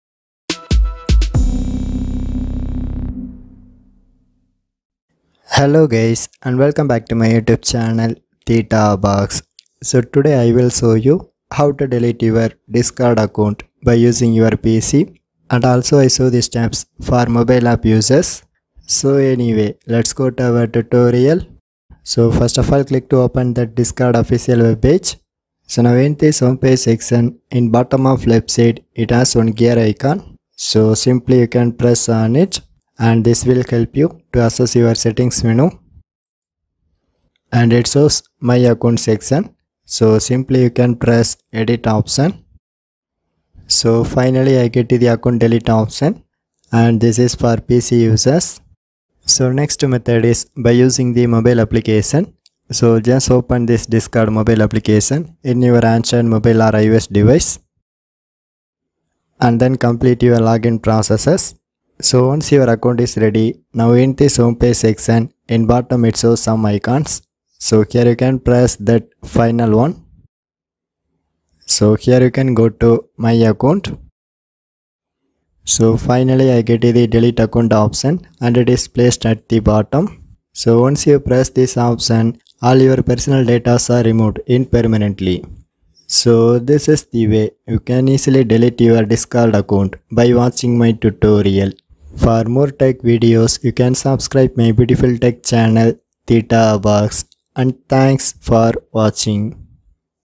How To Delete Discord Account (PC & Mobile)
This tutorial is about how to delete discord account permanently.

Once you deactivate it there is no possible to recover the deleted all messages.

If you get you own servers.In order to delete your account you must first transfer ownership of all servers that you own error message,first of all make all server into your own server.

Some user's are ask how long does it take to delete a discord account?.My answer is it takes more than 14 days.

Always use 2023 released new update of mobile app on your device like ios iphone or ipad.

This video has english subtitle at same time tamil,hindi,telugu and malayalam region people's are can easily understand it.

how to delete discord account without password :

1.Some user's are raising one question about how to get rid of my discord immediately?.

2.Here i show the steps by using my android phone and pc.

3.And this is short and faster method.

4.First method is for computer/laptop user's.

5.Just click to access that official webpage and click to login your account details.

6.But without logging in your id there is no possible to remove the profile.

7.In home page section you can press that gear icon to access the settings menu.

8.And then press "Edit" option,finally it has disable account and delete account options.

9.So you can choose any one of the option.

10.Once you press this link your id is instantly disabled in temporarily.